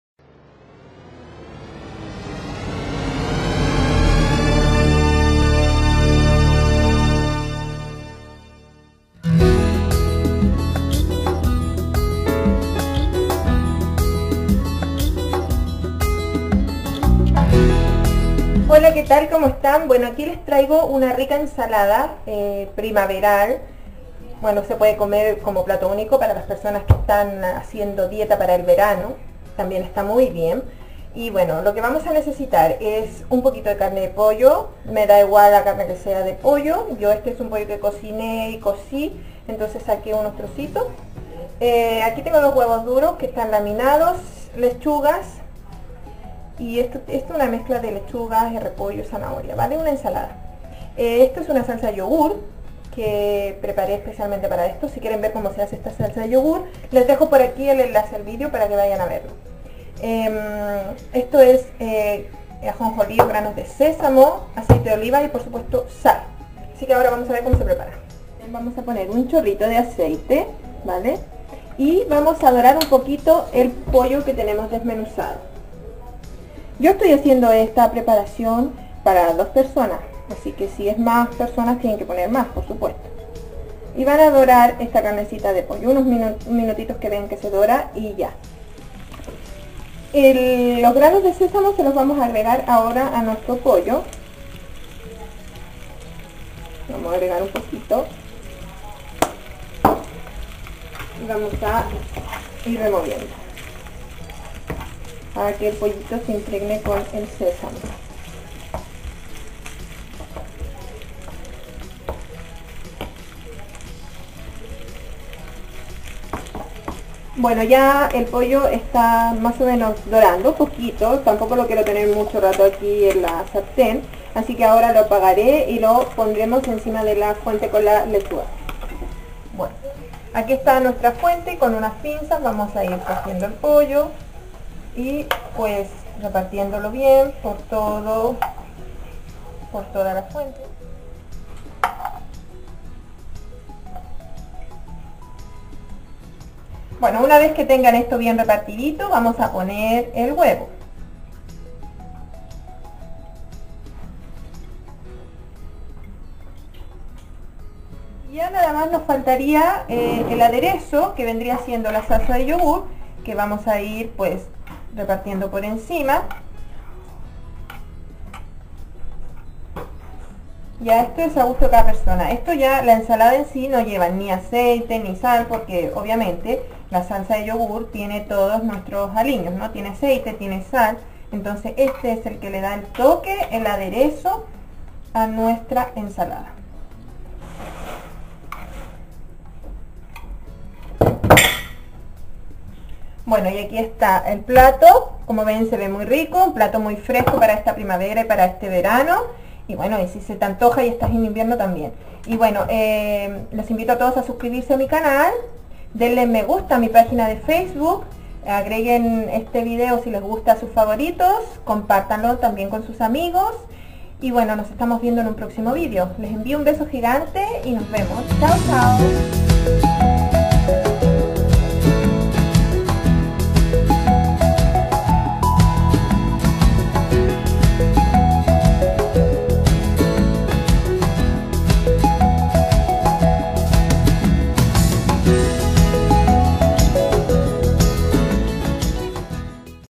Receta: Ensalada Fresca De Pollo Y Salsa De Yogur - Silvana Cocina Y Manualidades
Receta paso a paso para hacer deliciosa y fresca ensalada. Muy saludable y facil,  ideal para este verano!